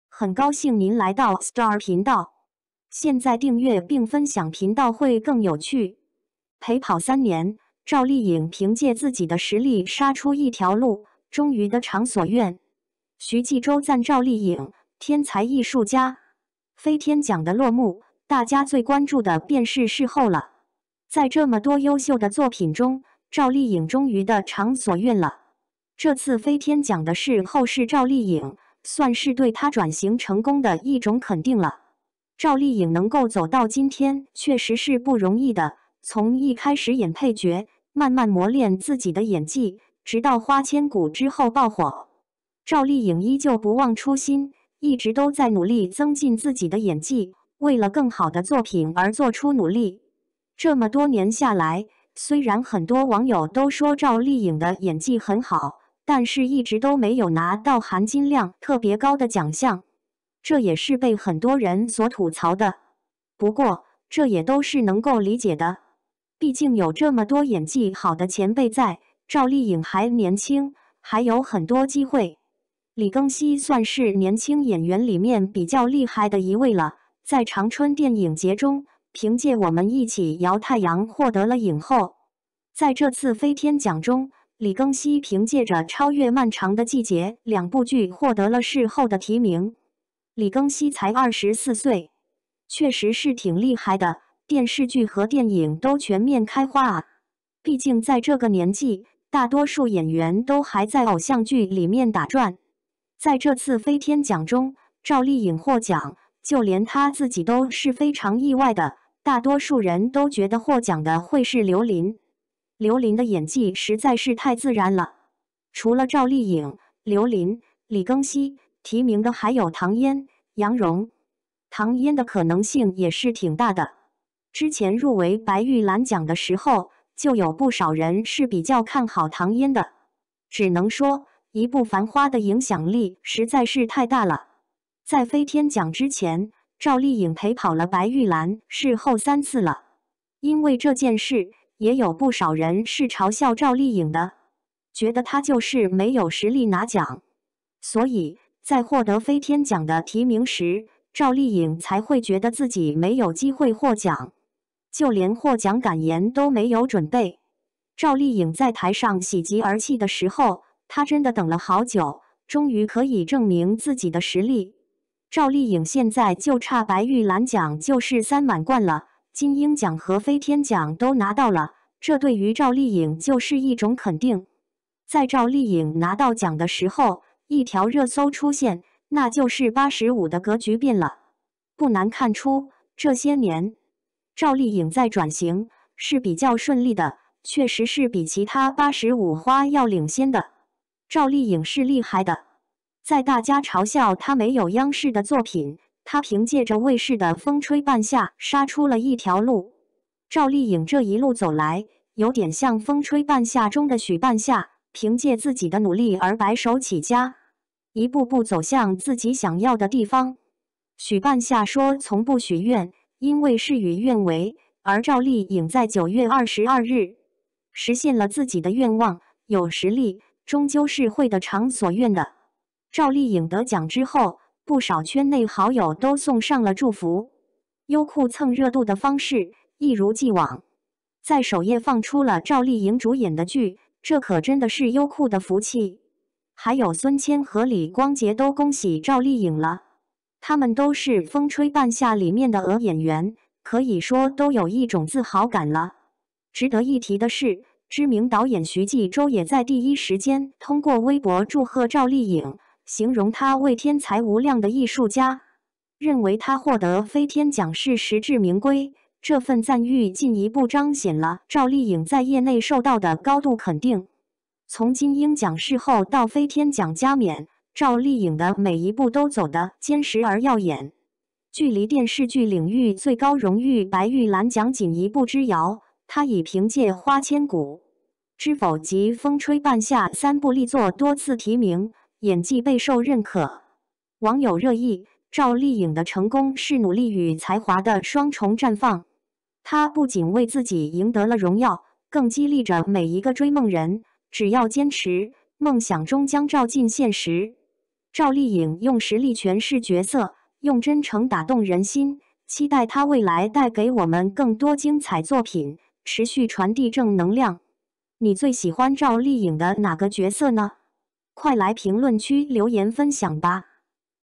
陪跑三年，赵丽颖凭借自己的实力杀出一条路，终于得偿所愿。徐纪周赞赵丽颖：天才艺术家
赵丽颖，1987年10月16日出生于河北省廊坊市，中国内地影视女演员。
2006年，获得《雅虎搜星》比赛冯小刚组冠军，从而进入演艺圈  。2011年，因出演古装剧《新还珠格格》开始崭露头角  。2013年，凭借古装剧《陆贞传奇》获得更多关注。2014年，在第10届中国金鹰电视艺术节投票活动中被选为“金鹰女神”  ；同年，凭借爱情剧《杉杉来了》获得第5届国剧盛典内地最具人气女演员奖。
2015年，主演的仙侠剧《花千骨》创下中国内地周播剧收视纪录  ，其个人也因此赢得广泛关注，更是凭借该剧先后获得第6届澳门国际电视节金莲花最佳女主角奖、第6届国剧盛典最具收视号召力演员奖、第22届上海电视节白玉兰奖最佳女主角奖提名、第28届中国电视金鹰奖观众喜爱的女演员奖  。2016年，担任河北省旅游形象大使 。2017年，凭借剧情片《乘风破浪》获得第24届北京大学生电影节最受大学生欢迎女演员奖  ；同年，其主演的古装剧《楚乔传》成为中国内地首部在播期间网络播放量突破400亿次的电视剧  。2018年，主演的古装剧《知否知否应是绿肥红瘦》播出，其个人则凭借该剧先后获得第25届上海电视节白玉兰奖最佳女演员提名  、第30届中国电视金鹰奖观众喜爱的女演员奖  。2022年，担任河北省影视家协会副主席  。